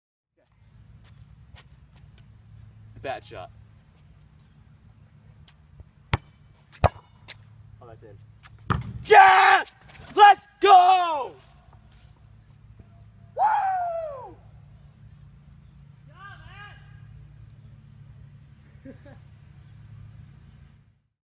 Base ball bat and basket ball
Skillful man uses baseball bat to whack basketball into net
Place for this in Rio? Skillful man uses baseball bat to whack basketball into net.. terrible practice of this youg boy has demonstrated.